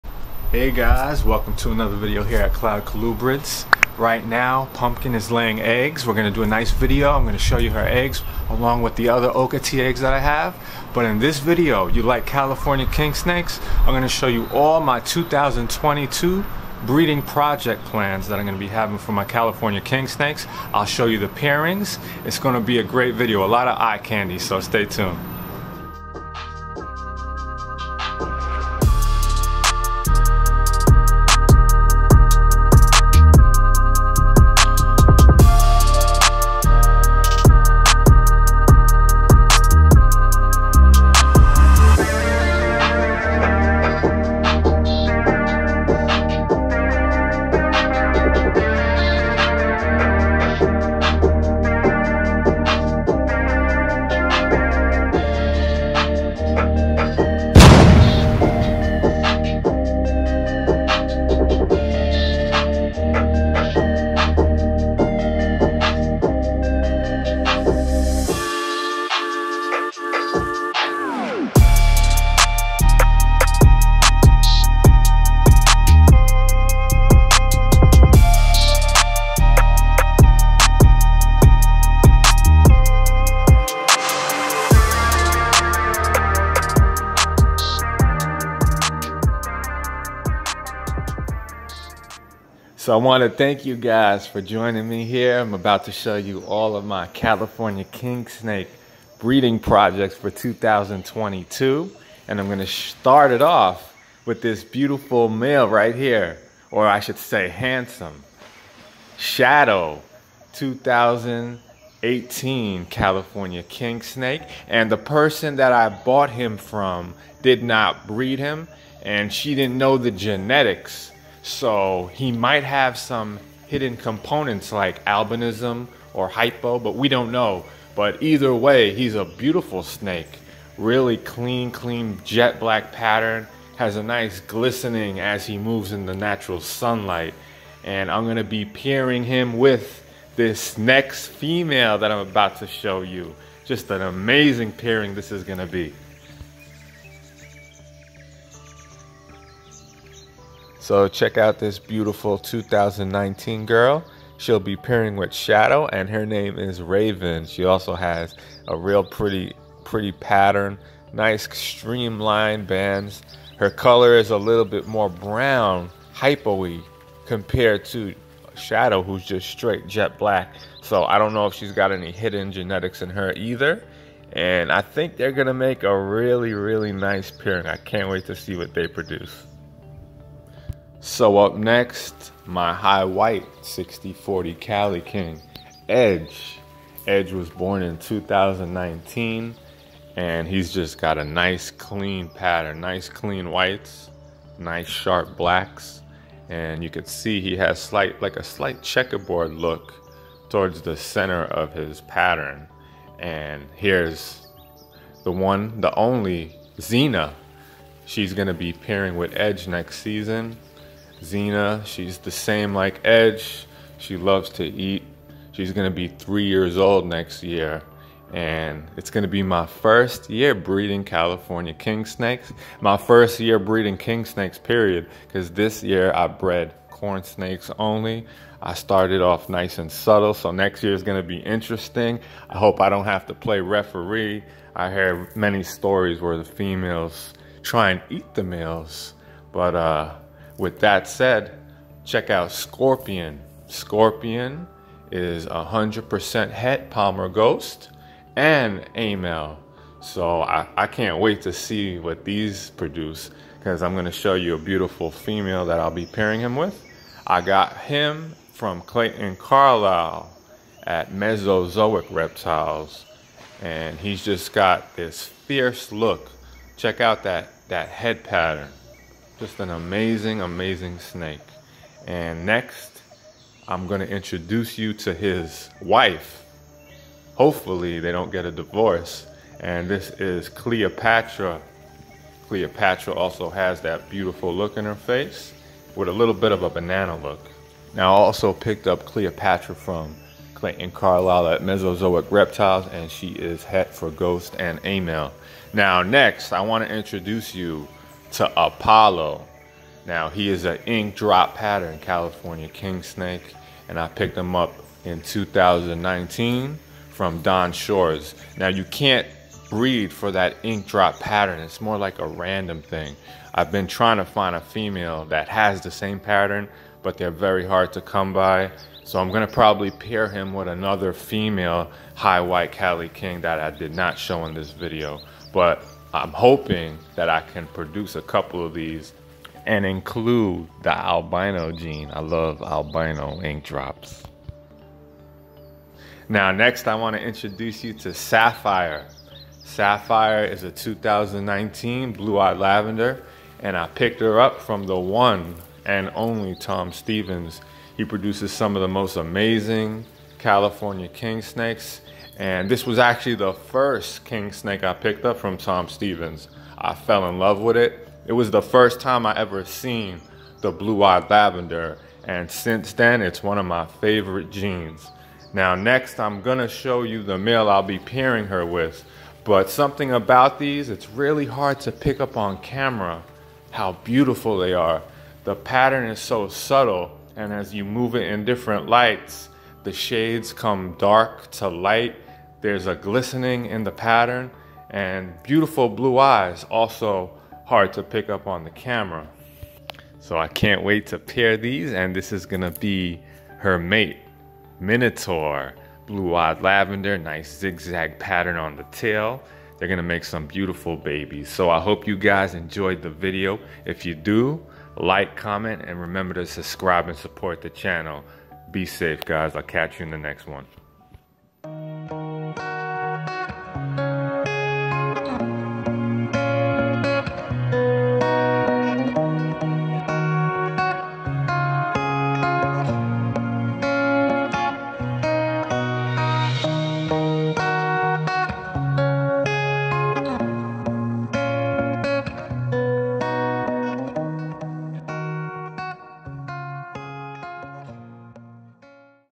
2022 Snake Breeding Projects (California kingsnakes)
In this video we will be showing you our
2022 snake breeding projects here at CloudColubrids.  California kingsnakes will be featured in this video. Kingsnakes are a large part of my snake collection and I cant wait to see what you think.  you have to see these snakes.